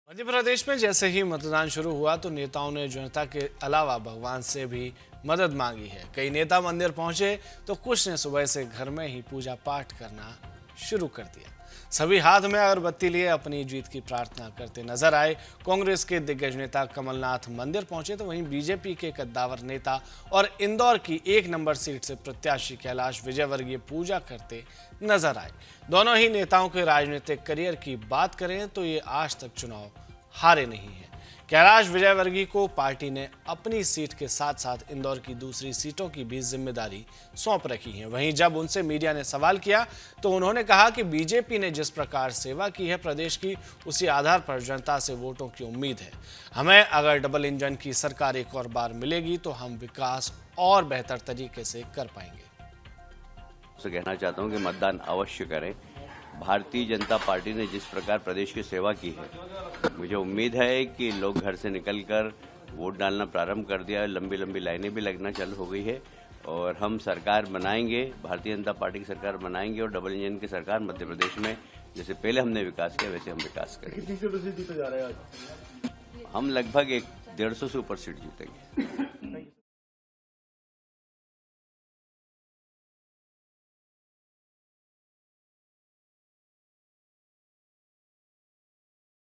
MP Election 2023: भगवान को प्रसन्न करने में जुटे नेता, आज मतदाता करेगा फैसला | MP News | Kamalnath
मध्य प्रदेश में विधानसभा चुनाव के लिए आज मतदान होगा। भाजपा और कांग्रेस दोनों पार्टियों के लिए यह चुनाव किसी अग्निपरीक्षा से कम नहीं होगा। मतदाताओं ने पिछली बार कमलनाथ की 15 महीने और शिवराज की साढ़े तीन साल की सरकार को देखा है। उसी आधार पर जनता अपना फैसला सुना सकती है। इस बार जहां कमलनाथ और दिग्विजय सिंह जैसे कांग्रेस के दिग्गज नेताओं का भविष्य दांव पर होगा तो वहीं भाजपा के केंद्रीय मंत्रियों और चार सांसदों के राजनीतिक भविष्य के लिए भी यह चुनाव अहम होगा। दोनों दलों प्रचार में कोई कसर नहीं छोड़ी. इसका नतीजा 3 दिसंबर को आएगा.